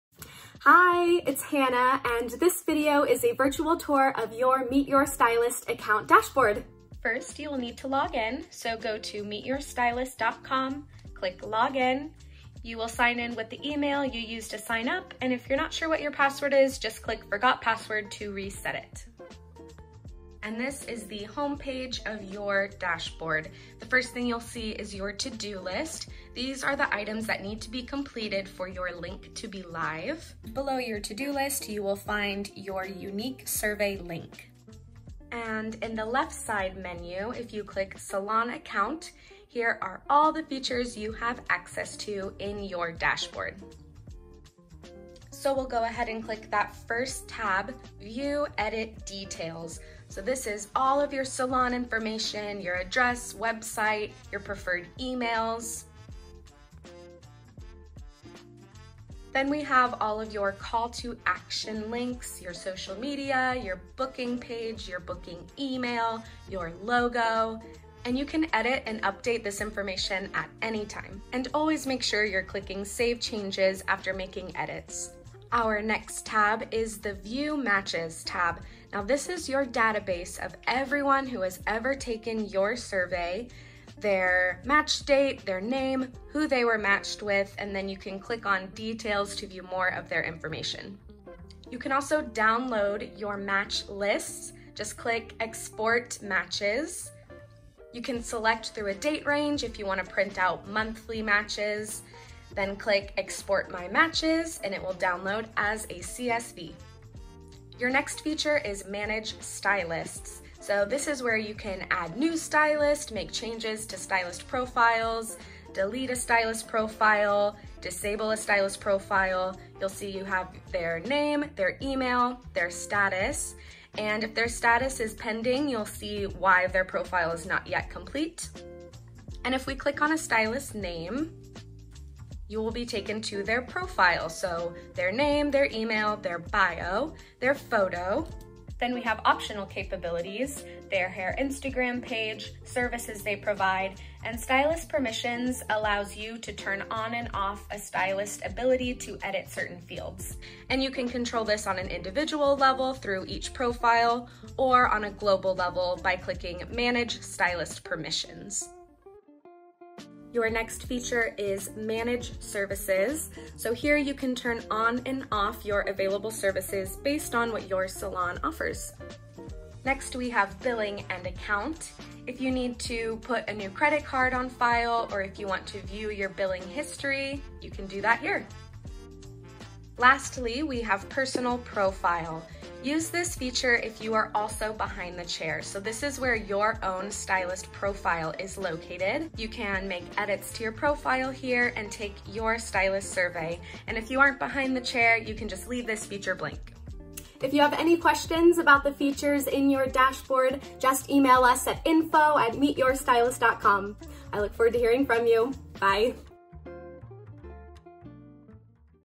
How to Access & Manage Your Dashboard
Here is a short tutorial on how to login to your account and maneuver through the dashboard. Our easy to use menu of options can help you add stylists, export match lists, and edit company permissions.